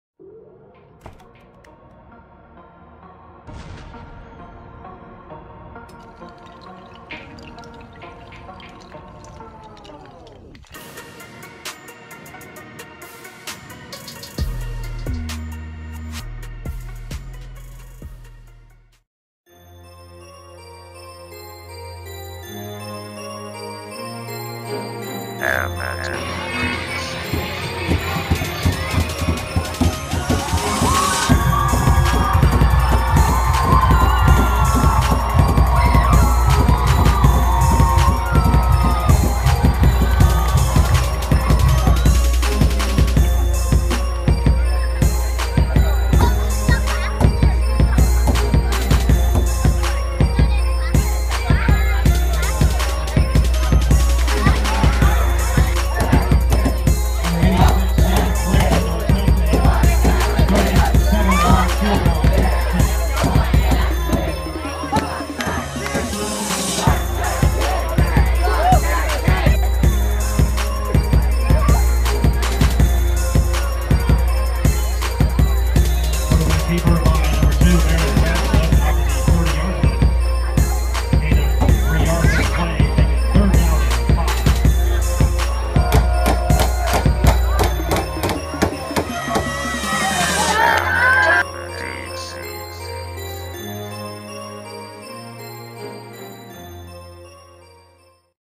Norcross High Vs Collins Hill
Norcross High school vs Collins Hill High school. Norcross hosted the game.